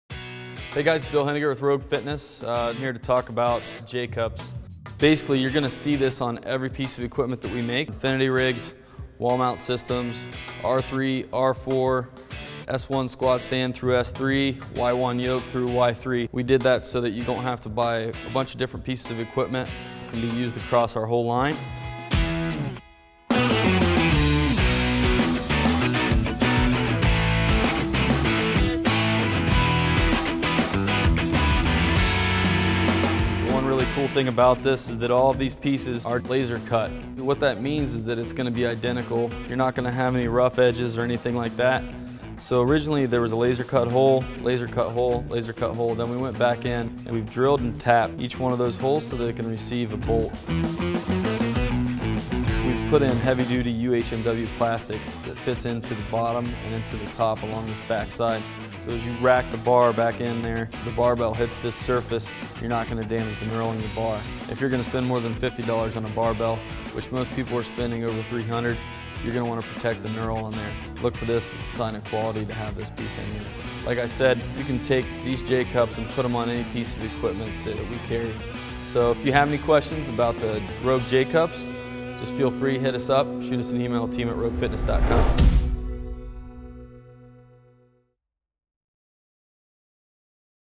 Bill talks J-Cup
Heavy Duty laser cut and precision bent design. UHMW plastic insert protects your bars knurling from damage.  New lower profile design with rounded corners for safety.  Bullnose 5/8" pin design for quick and easy adjustments.  These J-cups will support over 1000LB.

Dimensions:

6" height
4" depth
2" width